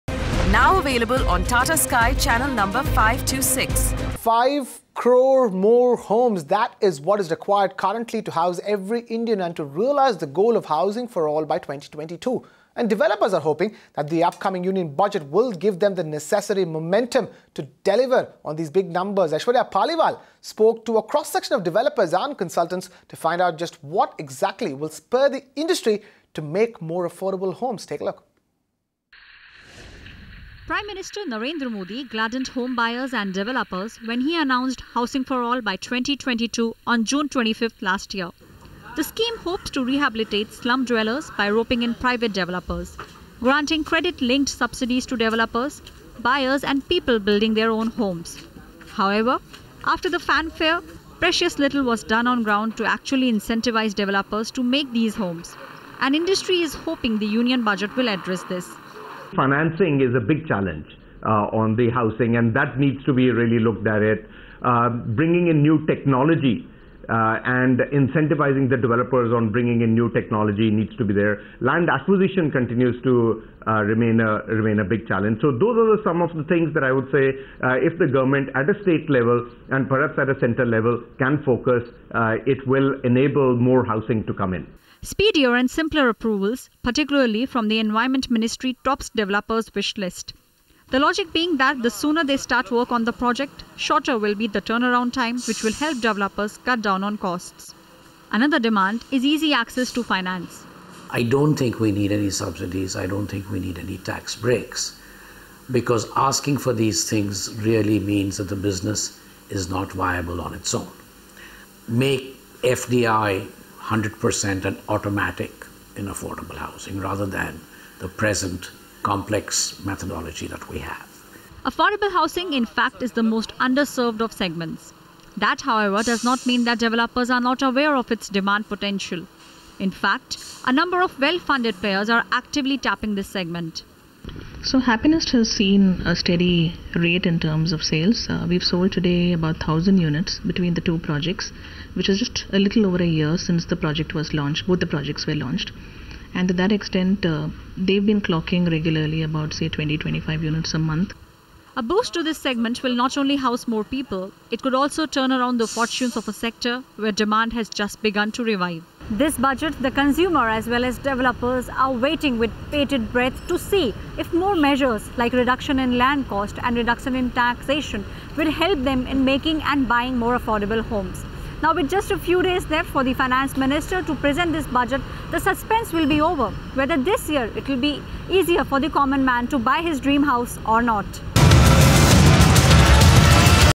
The News - Budget 2016 - What developers want for affordable housing
5 crore more homes are required to house every Indian and to realize the goal of Housing for all by 2022. Developers are hoping that the upcoming Union Budget will give them the necessary momentum to deliver these numbers. Tune in to the video to know more: